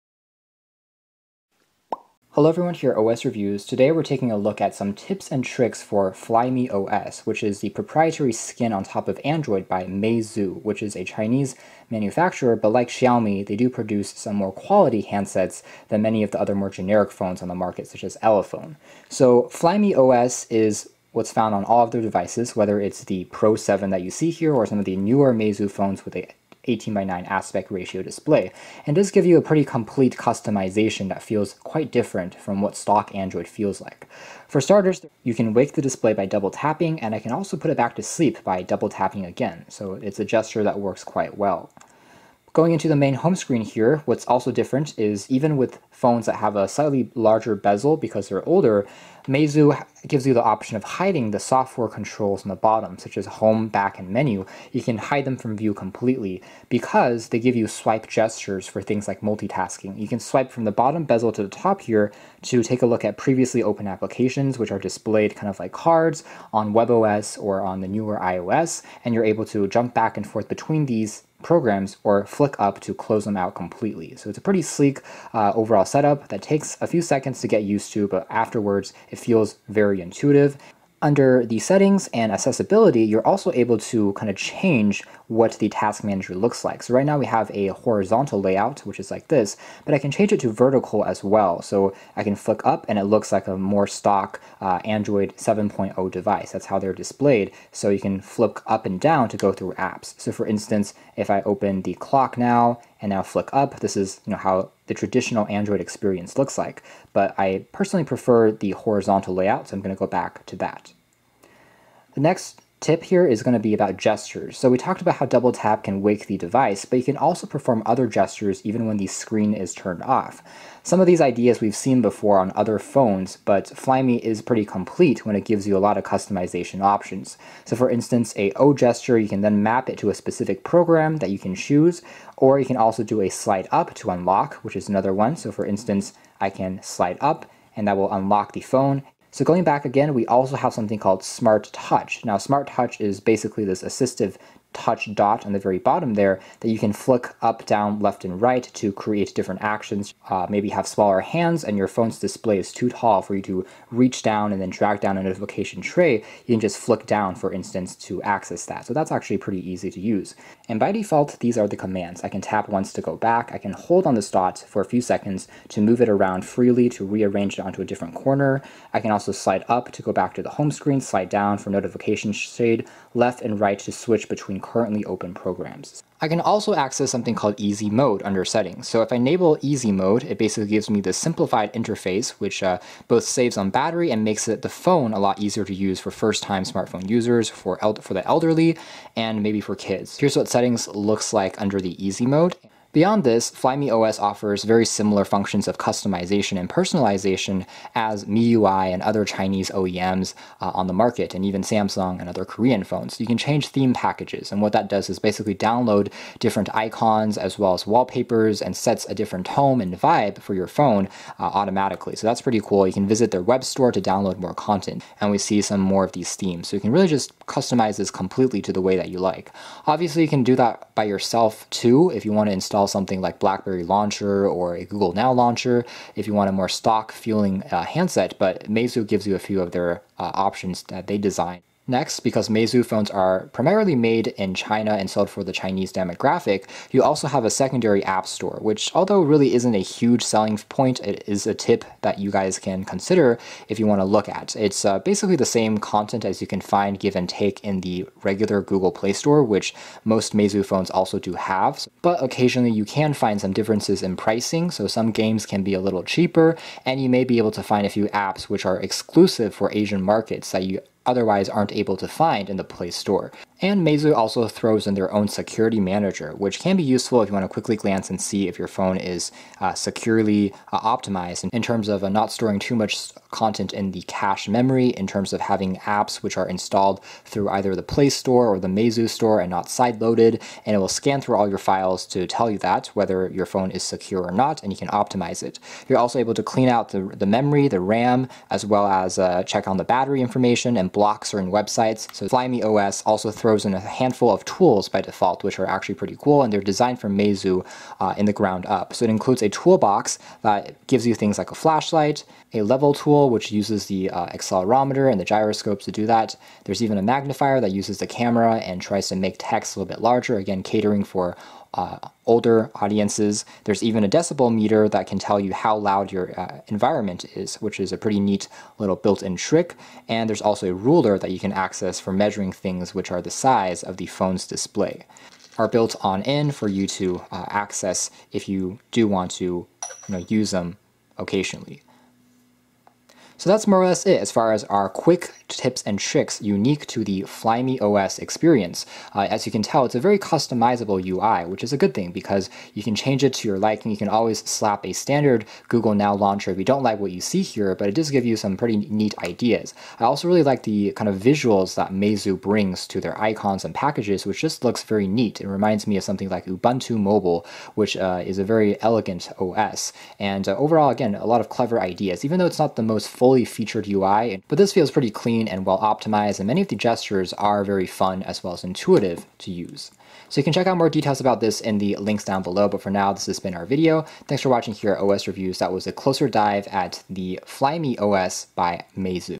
Flyme OS (Meizu): Tips & Tricks (Closer Look)
A closer look at software tips/tricks in Flyme OS

Details:
Flyme is a stock and aftermarket firmware developed by Meizu for smartphones based on the Android operating system. Some of its key features are completely redesigned apps, one-handed usability and performance optimizations. The current version of Flyme features distinguished flat design, various optimizations for performance. Flyme also has a feature called mBack, which is specific for Meizu smartphones. It allows easier navigation using only one physical button without on-screen navigation buttons. The back button function is implemented by tapping the physical button, while the home button function is used by pressing the physical button. The overview of recent application, which is on regular Android devices usually triggered by touching or tapping a dedicated button, can be viewed by swiping upwards from beneath the display.

The initial version, Flyme 1.0 based on Android 4.0.3 was released for the Meizu MX on June 12, 2012 and later for the Meizu M9. It was praised by critics for its simple yet beautiful interface design.

Flyme 2.0 was released later in 2012 together with the Meizu MX2. It brought improved cloud services and build-in root support, among other improvements.

Flyme 3.0 was released together with the Meizu MX3 and later for the Meizu MX2. It was one of the first Android firmware releases to integrate a flat UI. Since Flyme 3.0, Dirac HD Sound is integrated, bringing an improved audio experience through a certain number of ear and headphones.

The successor of Flyme 3.0, Flyme 4.0 was released in 2014, initially for the Meizu MX4. It was the first Flyme firmware to be made available on non-Meizu devices. Eventually, it was officially released for devices from Samsung, Sony, LG and HTC. Fans also ported it to other devices, including the OnePlus One. The Flyme4.5 is based on Android Lollipop and comes standard with the Meizu m2, Meizu m2 note and Meizu MX5. It was also made available for the Meizu MX4, Meizu MX4 Pro and Meizu m1 note.

Flyme 5 is based on either Android Lollipop or Android Marshmallow depending on the device. For Meizu devices, Flyme uses different editions for international devices ("G" release) and mainland Chinese devices ("A" release).

The latest version of Flyme is Flyme 6.1.3 based on Android Nougat, which is now available only for Meizu Pro 7. Flyme 6.1.0 is the most popular Flyme 6 version available for Meizu Pro 6 Plus, M3 Max, MX6 and other devices.